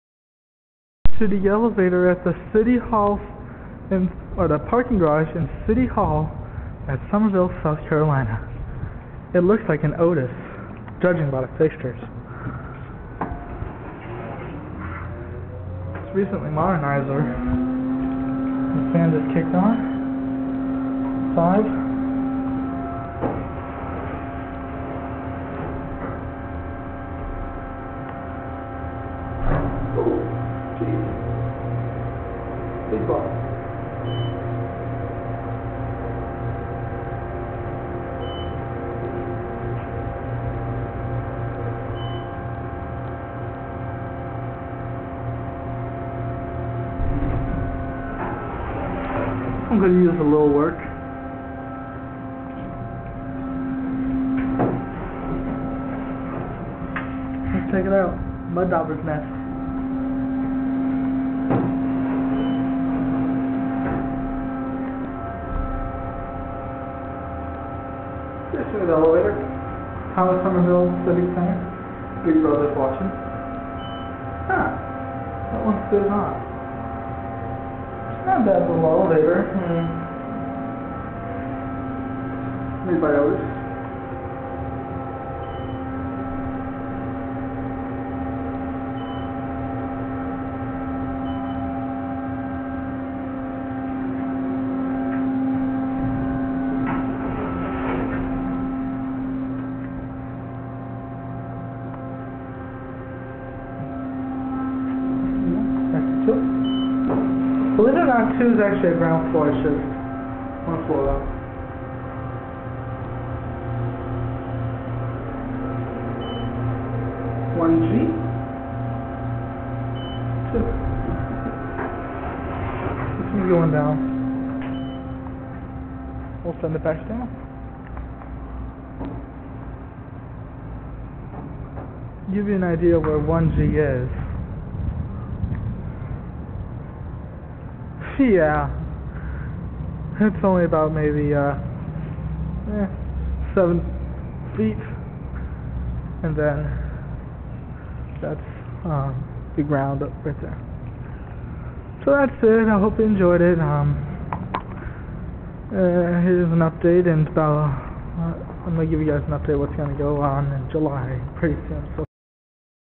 otis hydraulic with series 2 fixtures in summerville
this is at the parking garage in summerville these are very intersting elevators